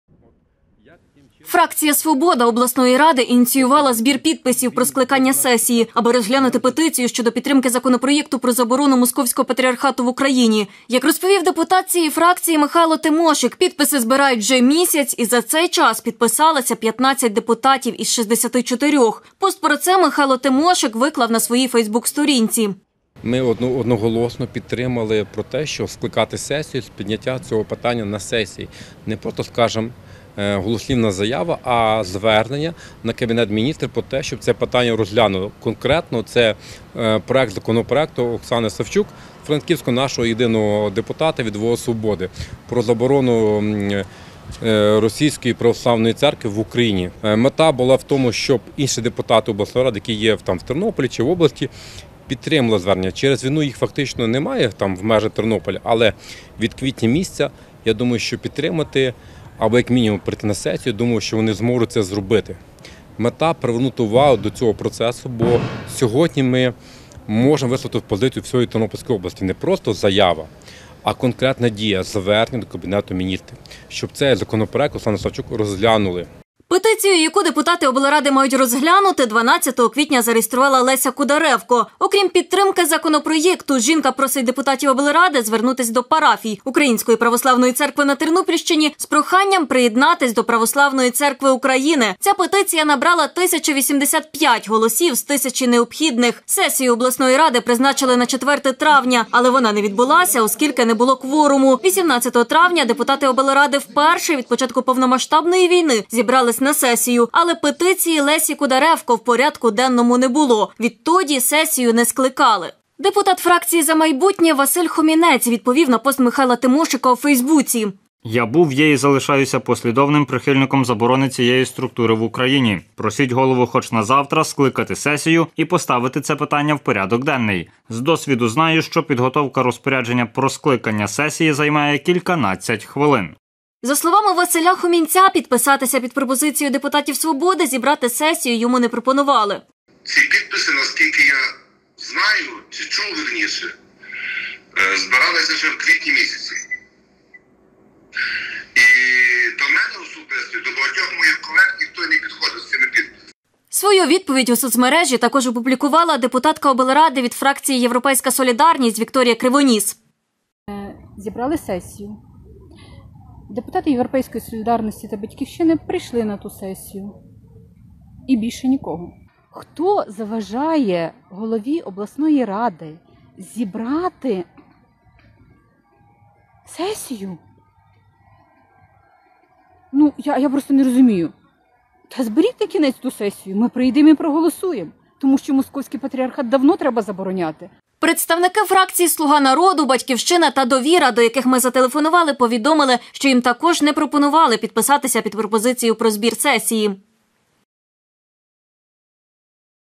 Заборона московського патріархату: фракція "Свобода" ініціює сесію Тернопільської обласної ради
Про це розповів Суспільному депутат цієї фракції Михайло Тимошик.